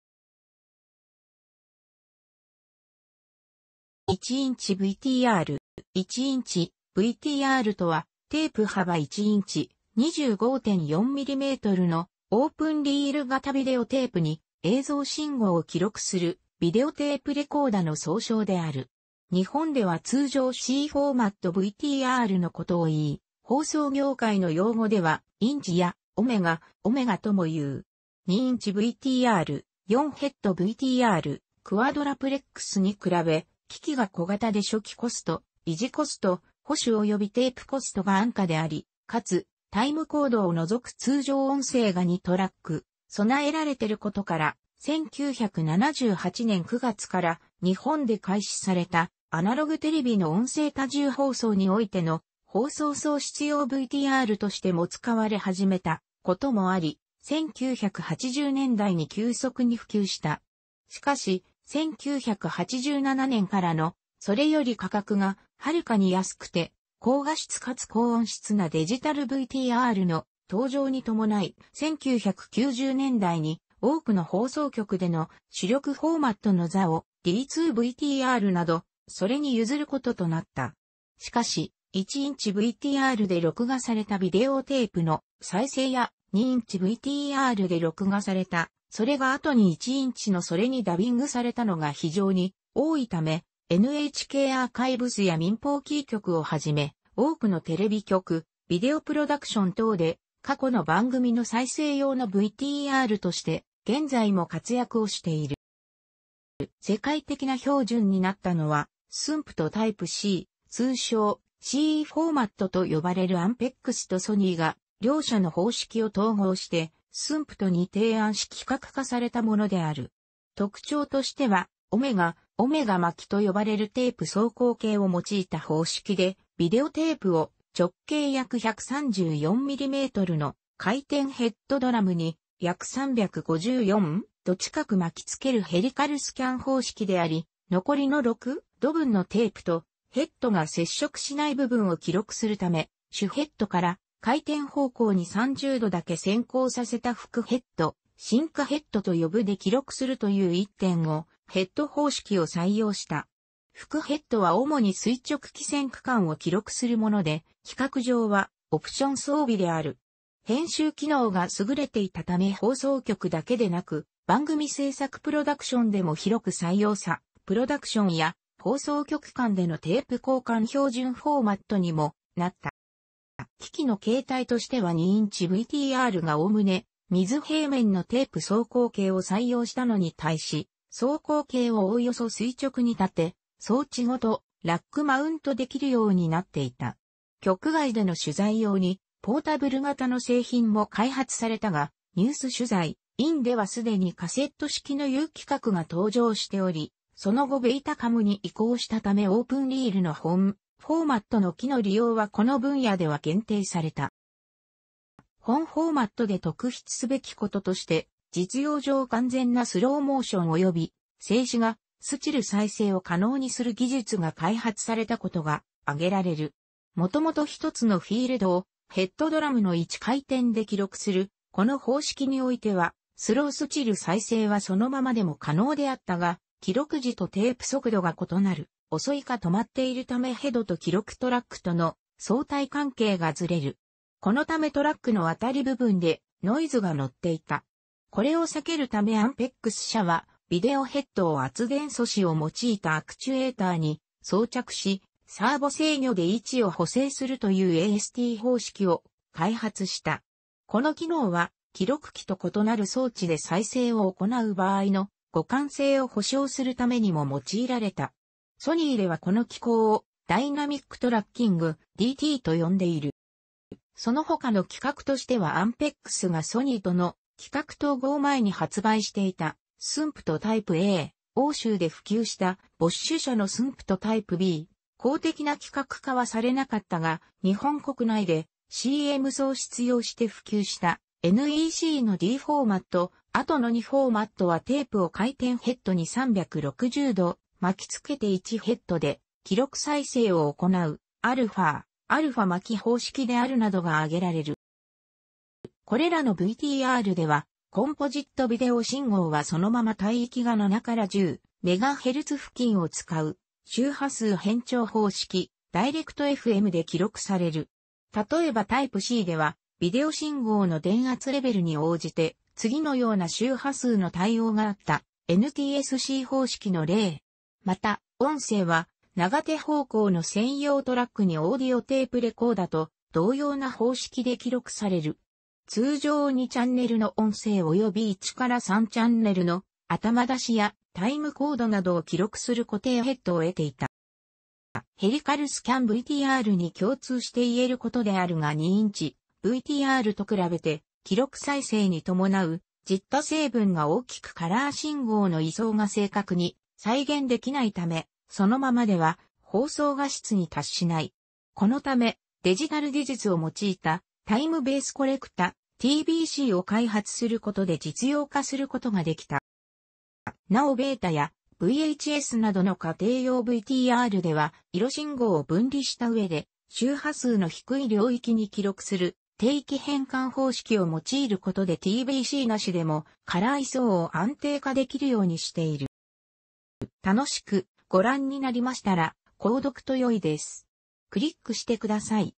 1インチVTR
1インチVTR

1インチVTRとはテープ幅1インチ（25.4mm）のオープンリール型ビデオテープに映像信号を記録するビデオテープレコーダの総称である。日本では通常CフォーマットVTRのことを言い、放送業界の用語では「インチ」や「オメガ（Ω）」ともいう。
2インチVTR（4ヘッドVTR, Quadruplex）に比べ、機器が小型で初期コスト・維持コスト（保守およびテープコスト）が安価であり、かつ、タイムコードを除く通常音声が2トラック備えられていることから、1978年9月から日本で開始された、アナログテレビの音声多重放送に於いての放送送出用VTRとしても使われ始めたこともあり(2インチVTRの音声は、1トラックのみのモノ仕様)、1980年代に急速に普及した。しかし、1987年からの、それより価格がはるかに安くて高画質かつ高音質なデジタルVTRの登場に伴い、1990年代に多くの放送局での、主力フォーマットの座を、D2-VTRなどそれに譲ることとなった。
しかし、1インチVTRで録画されたビデオテープの再生や、2インチVTRで録画されたそれが後に1インチのそれにダビングされたのが非常に多いため、NHKアーカイブスや民放キー局を始め、多くのテレビ局、ビデオ・プロダクション等で、過去の番組の再生用のVTRとして、現在も活躍をしている。

世界的な標準になったのはSMPTE TYPE-C、通称「Cフォーマット」と呼ばれるアンペックスとソニーが両社の方式を統合してSMPTEに提案し規格化されたものである。特徴としては「Ω（オメガ）巻き」と呼ばれるテープ走行系を用いた方式でビデオテープを直径約134mmの回転ヘッドドラムに約354?度近く巻きつけるヘリカルスキャン方式であり、残りの6?度分のテープとヘッドが接触しない部分を記録するため主ヘッドから回転方向に30度だけ先行させた副ヘッド（シンクヘッドと呼ぶ）で記録するという1.5ヘッド方式を採用した。副ヘッドはおもに垂直帰線区間を記録するもので、規格上はオプション装備である。編集機能が優れていたため放送局だけでなく番組制作プロダクションでも広く採用され、プロダクションや放送局間でのテープ交換標準フォーマットにもなった。

機器の形態としては2インチVTRがおおむね水平面のテープ走行系を採用したのに対し走行系をおおよそ垂直に立て、装置ごとラックマウントできるようになっていた。局外での取材用にポータブル型の製品も開発されたがニュース取材（ENG）ではすでにカセット式のU規格が登場しており、その後BETACAMに移行したためオープンリールの本フォーマットの機器の利用はこの分野では限定された。

本フォーマットで特筆すべきこととして、実用上完全なスローモーションおよび静止画（スチル）再生を可能にする技術が開発されたことが挙げられる。もともと一つのフィールドをヘッドドラムの1回転で記録するこの方式においてはスロー・スチル再生はそのままでも可能であったが、記録時とテープ速度が異なる（遅いか止まっている）ためヘッドと記録トラックとの相対関係がずれる。このためトラックの渡り部分でノイズが乗っていた。これを避けるためAMPEX社はビデオヘッドを圧電素子を用いたアクチュエーターに装着し、サーボ制御で位置を補正するというAST方式を開発した。この機能は記録機と異なる装置で再生を行う場合の互換性を補償するためにも用いられた。ソニーではこの機構をダイナミックトラッキング（DT）と呼んでいる。

そのほかの規格としてはアンペックスがソニーとの規格統合前に発売していたSMPTE TYPE-A、欧州で普及したBosch社のSMPTE TYPE-B、公的な規格化はされなかったが日本国内でCM送出用として普及したNECのDフォーマット（後の2フォーマットはテープを回転ヘッドに360度巻きつけて1ヘッドで記録再生を行う「α（アルファ）巻き」方式である）などがあげられる。

これらのVTRではコンポジットビデオ信号はそのまま帯域が7〜10MHz付近を使う周波数変調方式（ダイレクトFM）で記録される。例えばTYPE-Cでは、ビデオ信号の電圧レベルに応じて次のような周波数の対応があった（NTSC方式の例）。
また、音声は長手方向の専用トラックにオーディオテープレコーダと同様な方式で記録される。通常2チャンネルの音声および1〜3チャンネルの頭出しやタイムコードなどを記録する固定ヘッドを備えていた。

ヘリカルスキャンVTRに共通して...